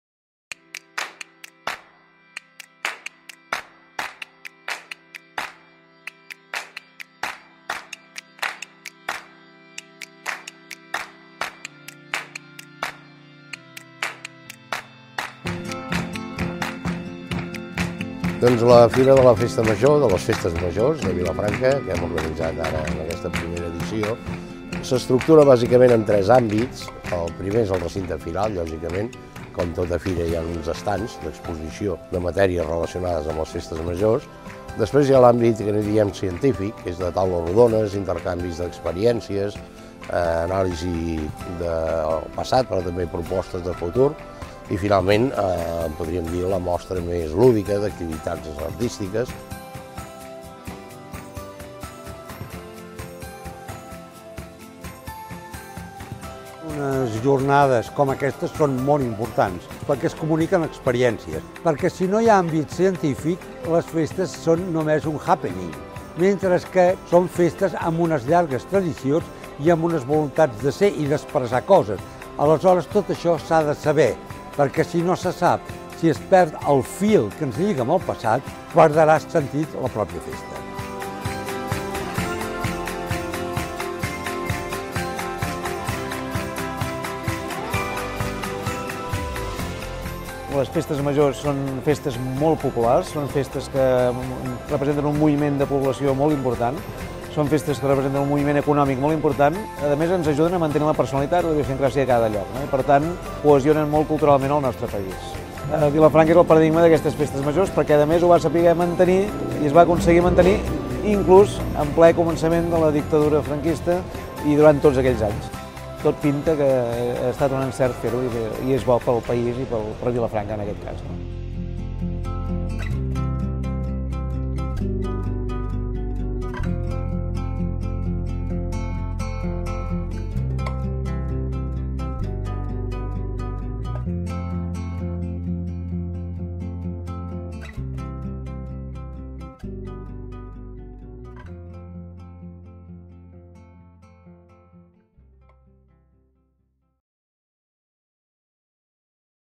I Fira de les Festes Majors.
Del 29 d'octubre a 1 de novembre de 2015, Vilafranca del Penedès celebra la I Fira de les festes majors, amb un programa dividit en els àmbits científic, firal i artístic.